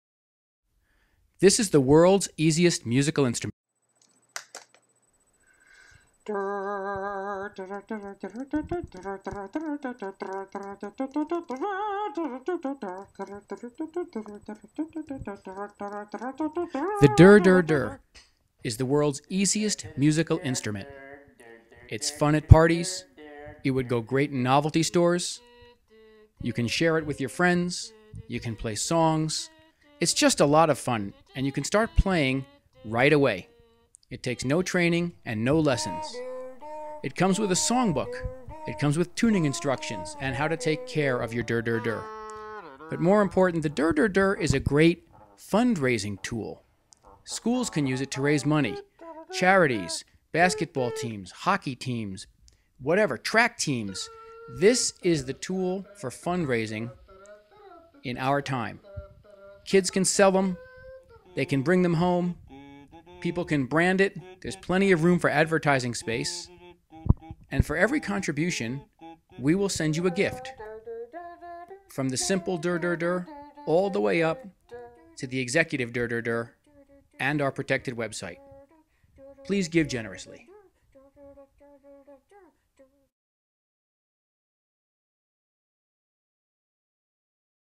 Easiest Musical Instrument - Fun and Simple a Great Fundraiser
This is a fun easy instrument. It comes with a songbook sheet music and tuning instructions. A Great gift